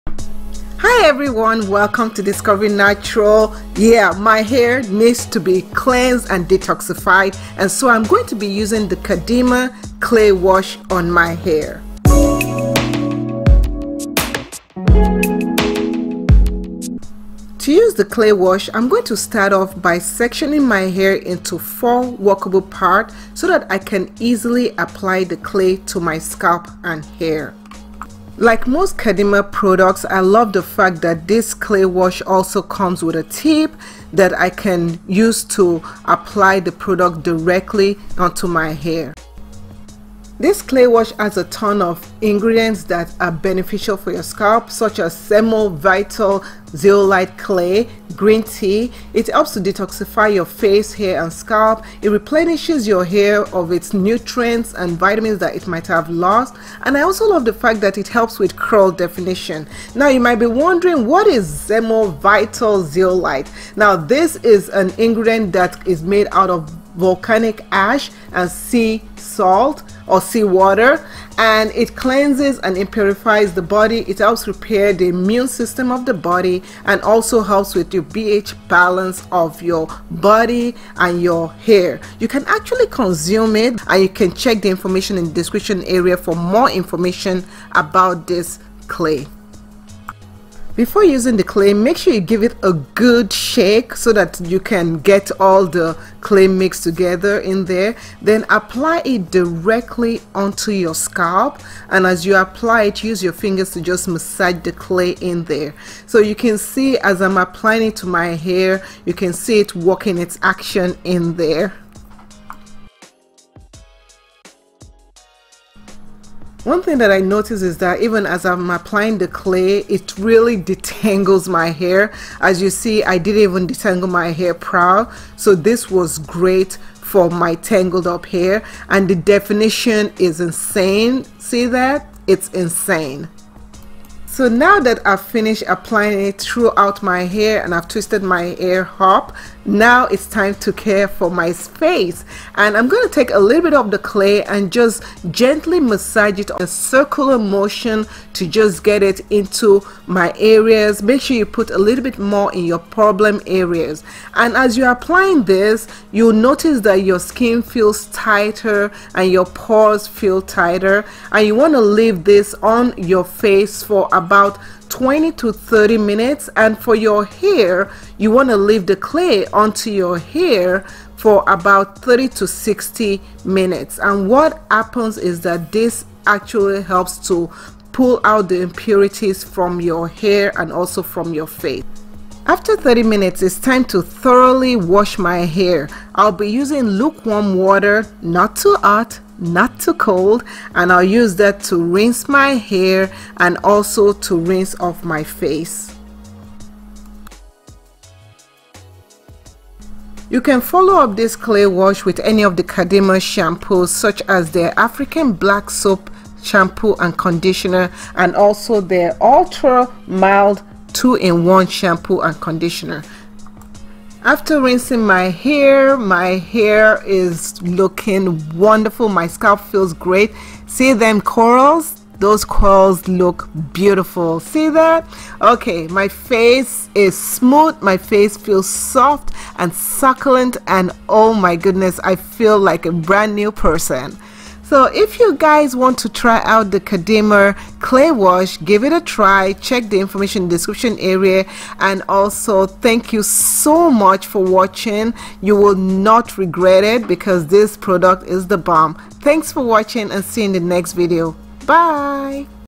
How to Detox Your Scalp and Hair with Kadima Clay Wash
When my scalp and hair needs detoxing, I use Kadima Organic Beauty Products Clay Wash. This contains SomaVital Zeolite Clay made of Volcanic ash and sea water. This clay replenishes lost nutrients and vitamins that the hair and body needs.